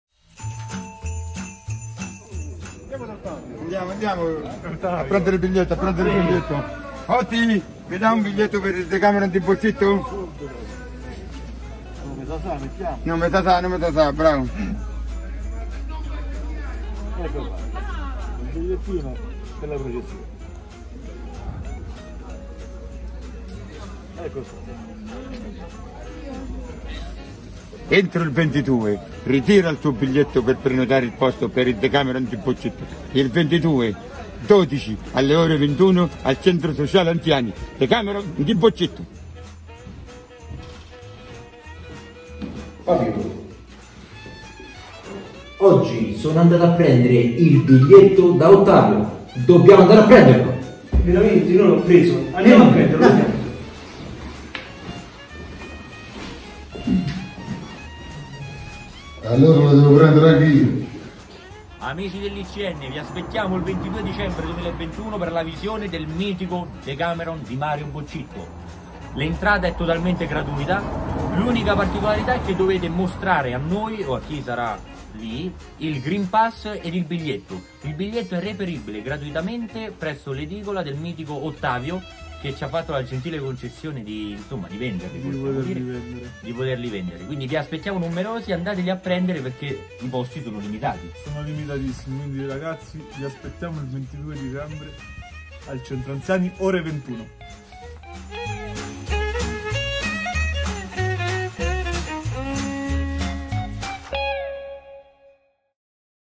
I.C.N. Spot Biglietti
Dopo il lancio del trailer ecco lo spot ufficiale, andate a prendere i biglietti dal mitico Ottavio per la serata evento del 22/12